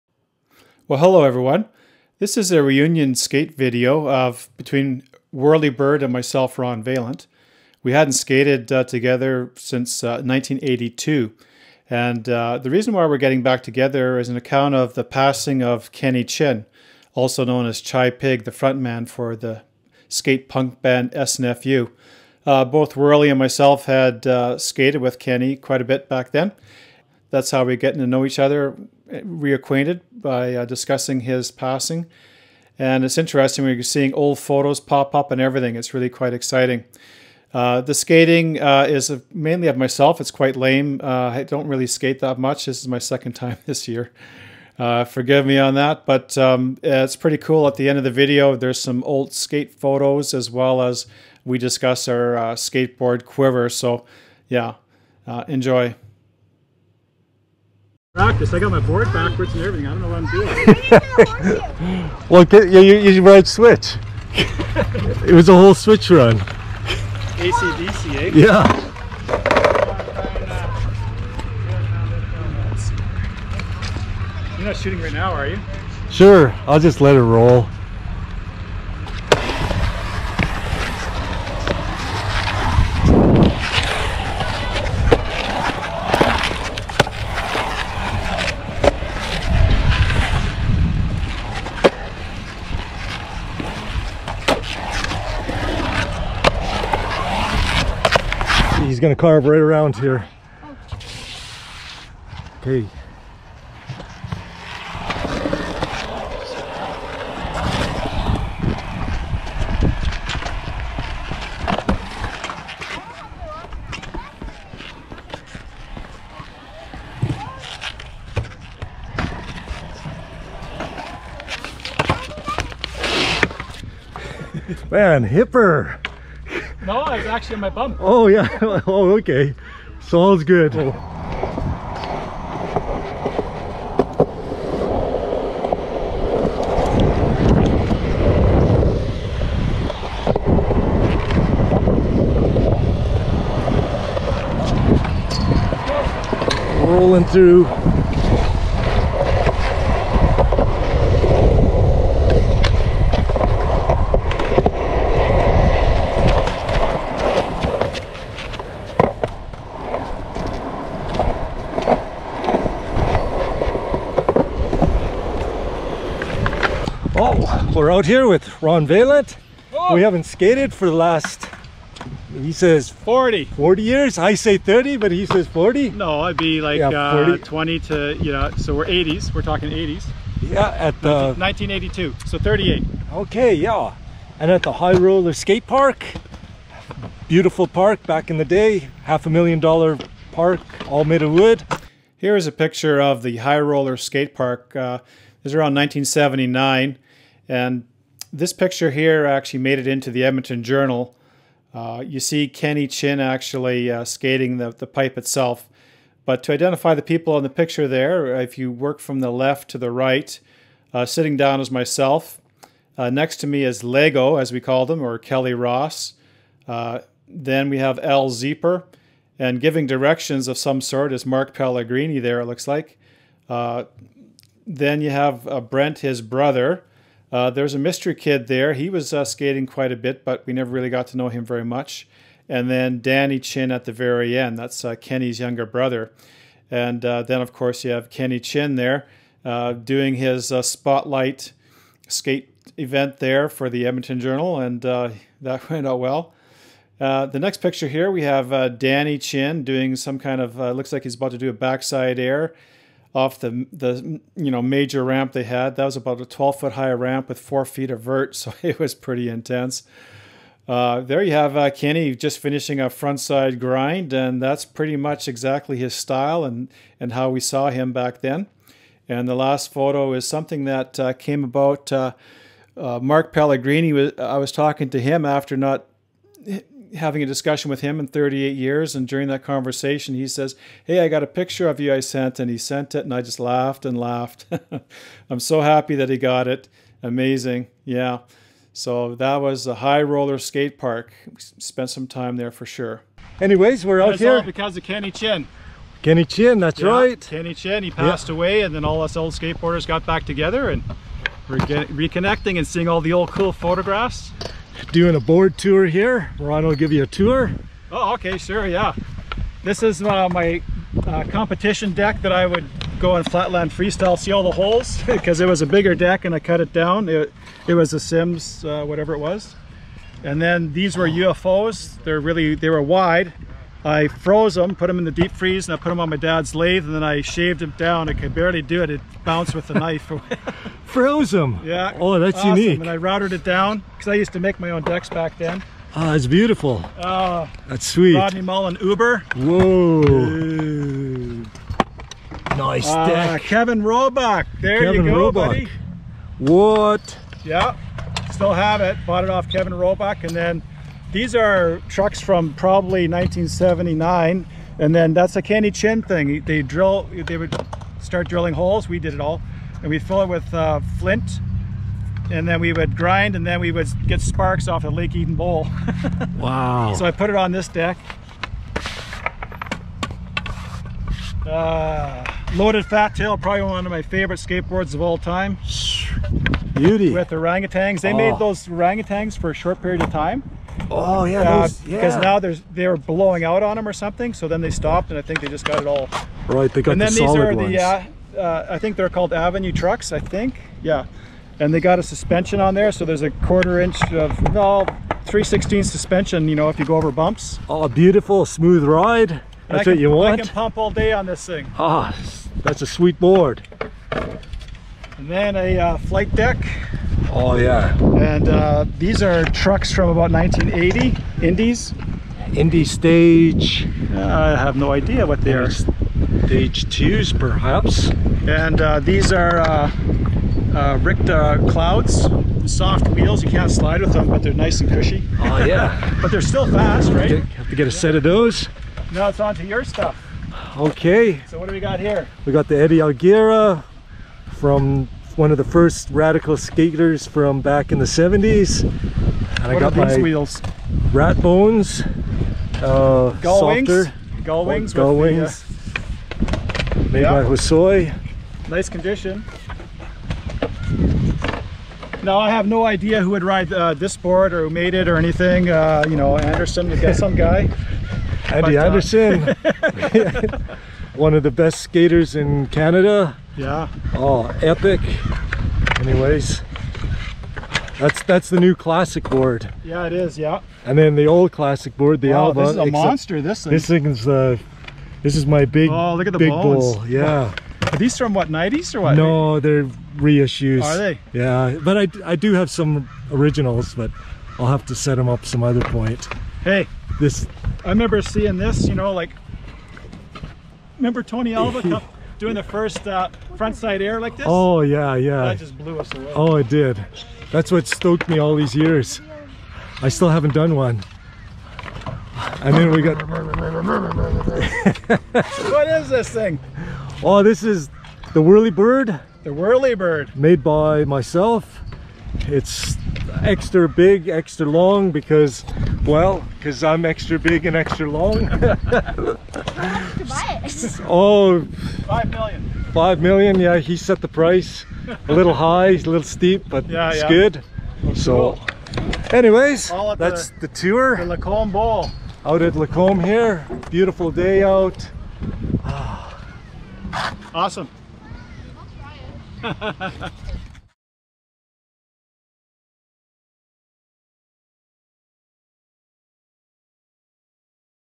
Chi Pig Skate Reunion - Lacombe, Alberta, Canada Skateboard Park.
Kenny Chin, Chi Pig, of the Skate Punk Band SNFU passed away July 16 2020. Wurly Byrd and myself, Ron Vaillant, spent time skating with Kenny and his younger brother Danny.

Kenny's death has brought a lot of us old skaters back together through social media. People I have not seen, or heard from, in over 30 years have come out of the wood work. This has brought forth a New Life of sorts in old friendships.

This video is my reunion with Wurly Byrd after a 38 year hiatus. It was being part of a very competitive skate community that we were able to excel way farther than had we skated alone. It's one of the reasons I do not go to parks very often. With old reunions, it looks like there might be people to skate with after all. Thanks Ken. SNFU. KennyChinn Old Man Skateboarder, Skater, at the Skate Park.